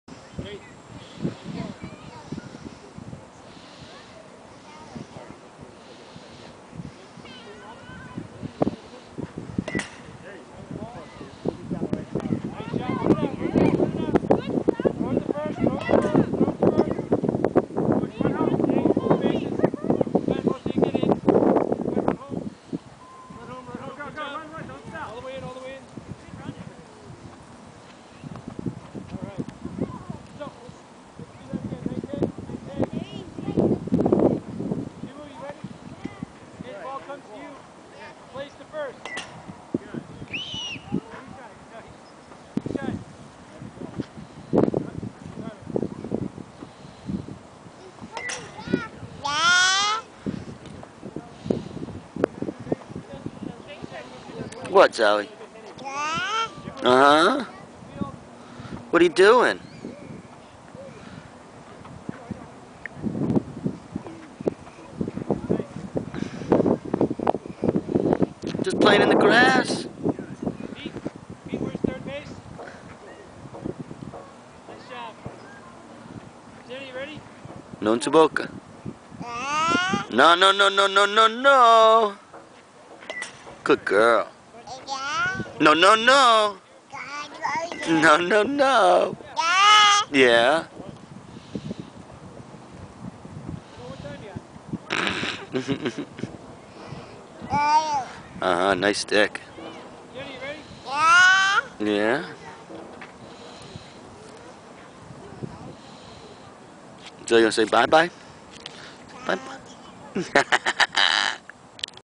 zoe Herzog donaldson park
Zoe in the park on first spring like day